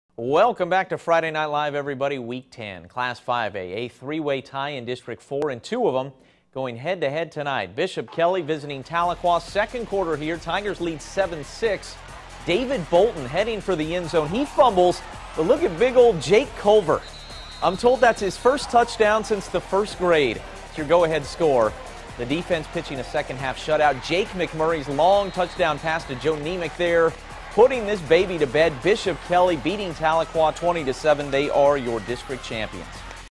Bishop Kelley Comets vs Tahlequah Tigers - Oklahoma High School Football
Highlights of the Bishop Kelley, Tahlequah High School Football Game